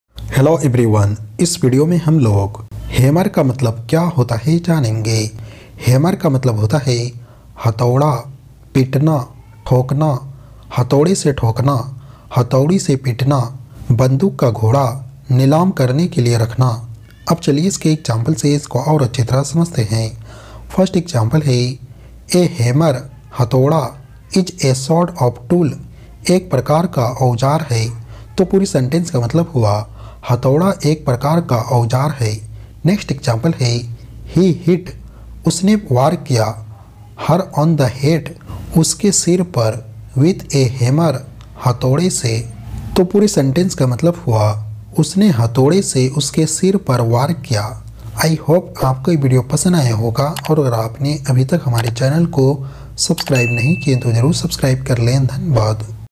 Hammer Meaning in Hindi | Hammer ka Matlab kya hota hai | Word Meaning English to Hindi
Hammer Meaning in Hindi | Meaning of Hammer | Hammer का हिंदी में मतलब | English to Hindi vocabulary |

Aaj ki Is Video Me Maine Aapko Hammer Ka Matlab Kya hota hai Hindi mein, Hammer Ka definition or Hammer Word Ke Sentence ka Example ko batlaya hai.